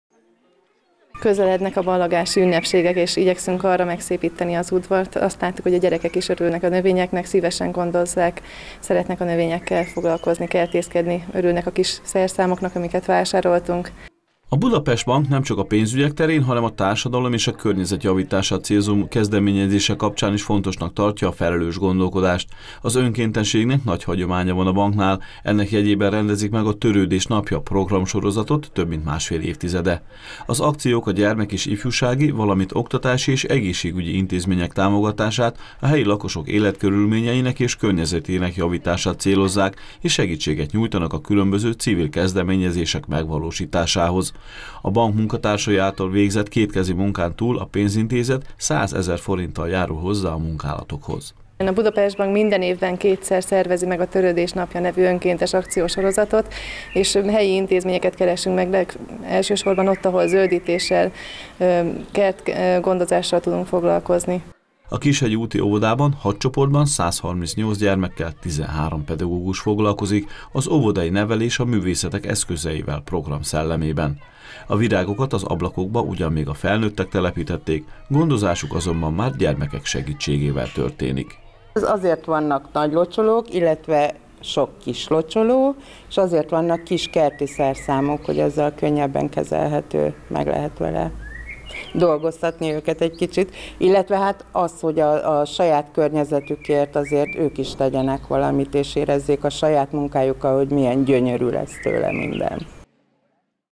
A Budapest Bank virágosított a paksi óvodában
A Törődés Napja önkéntes akció keretében a paksi Benedek Elek Óvoda Kishegyi úti tagóvodájának kertjét parkosították a Budapest Bank paksi fiókjának önkéntesei. Az eseményt a pénzintézet idén tavasszal és nyáron országszerte 24 helyszínen rendezi meg.